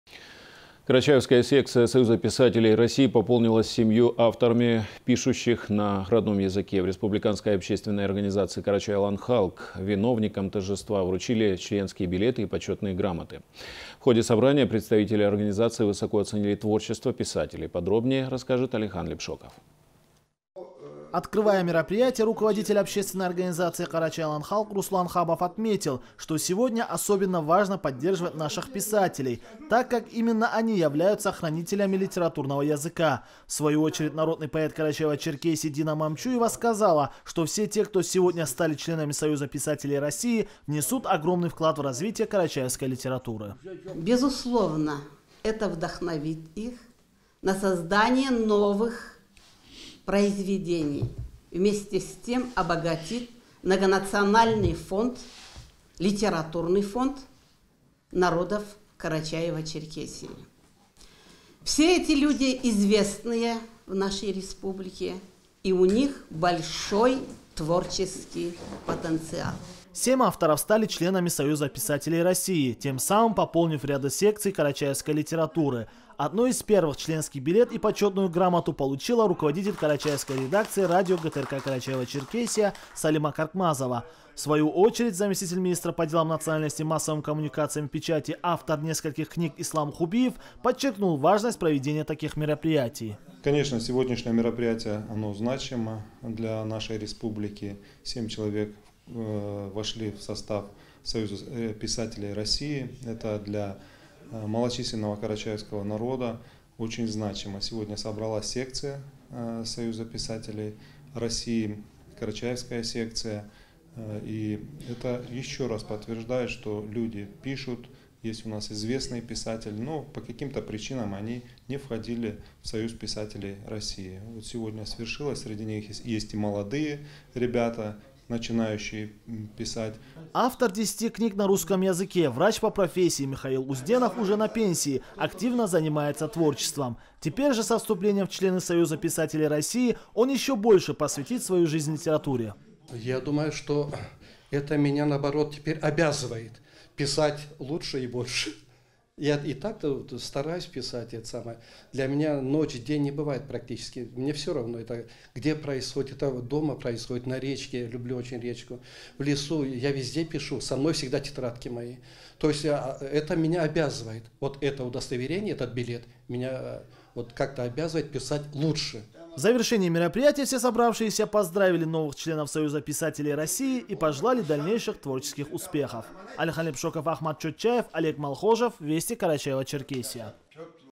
Карачаевскую секцию Союза писателей России пополнили 7 авторов, пишущих на родном языке
Карачаевская секция Союза писателей России пополнилась 7-ю авторами, пишущими на родном языке. В республиканской общественной организации "Къарачай-Алан Халкъ" виновникам торжества вручили членские билеты и почетные грамоты